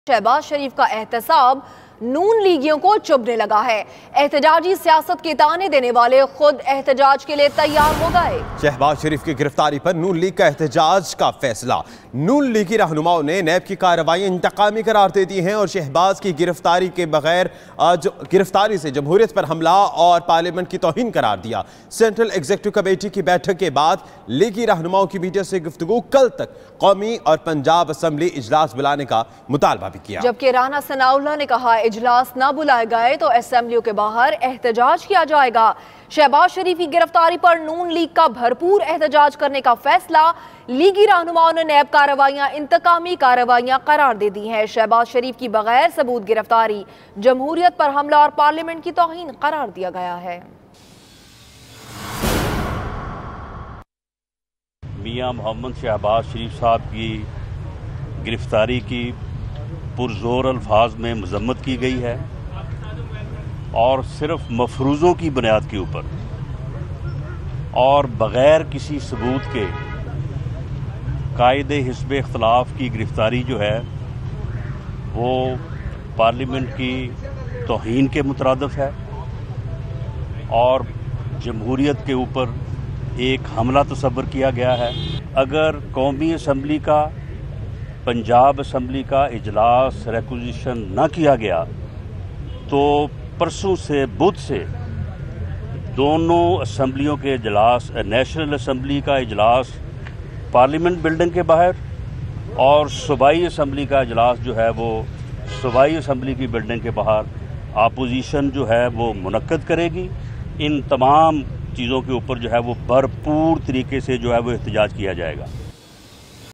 PML-N threatens to protest if Shehbaz not allowed to attend NA, PA sessions | 8 Oct 2018
Watch: PML-N threatens to protest if Shehbaz not allowed to attend NA, PA sessions | 8 Oct 2018 | 92NewsHD